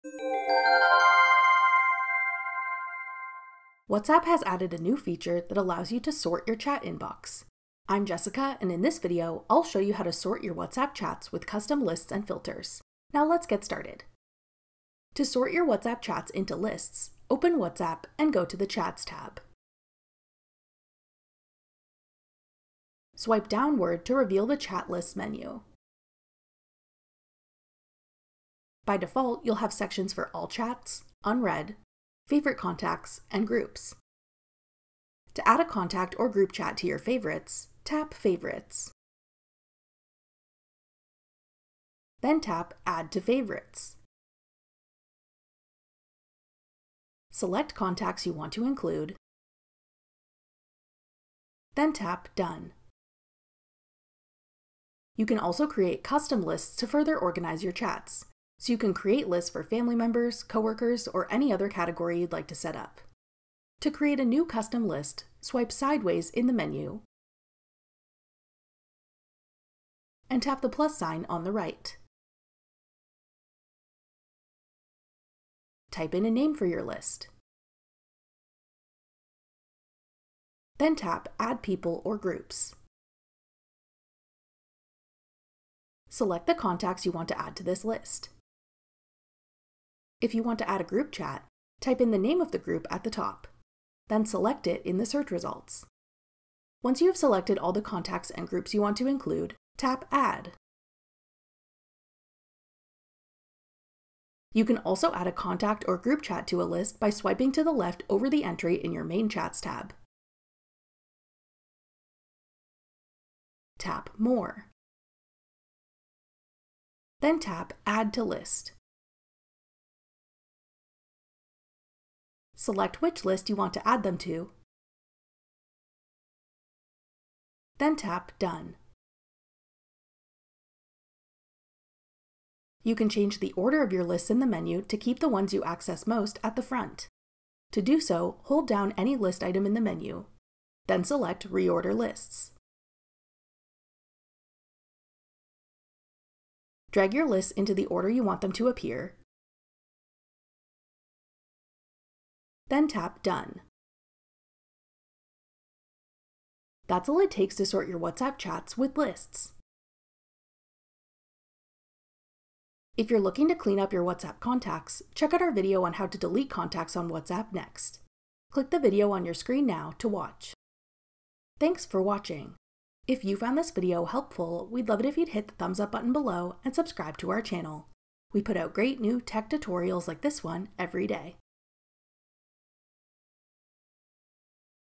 How to Sort WhatsApp Chats with Custom Lists (Full Guide)
WhatsApp has added a new feature that allows you to sort your chat inbox. Watch this video to learn how to sort your WhatsApp chats with custom lists and filters.

To sort your WhatsApp chats into lists, open WhatsApp and go to the Chats Tab. Swipe downward to reveal the chat lists menu. By default, you will have sections for All chats, unread, favourite contacts, and groups. To add a contact or group chat to your Favorites, tap Favorites. Then tap Add to Favorites. Select contacts you want to include, then tap Add.

You can also create custom lists to further organize your chats, so you can create lists for family members, coworkers, or any other category you’d like to set up. To create a new custom list, swipe sideways in the menu and tap the plus sign on the right. Type in a name for your list. Then tap Add People or Groups. Select the contacts you want to add to this list. If you want to add a group chat, type in the name of the group at the top, then select it in the search results. Once you have selected all the contacts and groups you want to include, tap Add.

You can also add a contact or group chat to a list by swiping to the left over the entry in your main chats tab. Tap More. Then tap Add to List. Select which list you want to add them to, then tap Done.

You can change the order of your lists in the menu to keep the ones you access most at the front. To do so, hold down any list item in the menu, then select Reorder Lists. Drag your lists into the order you want them to appear, then tap Done.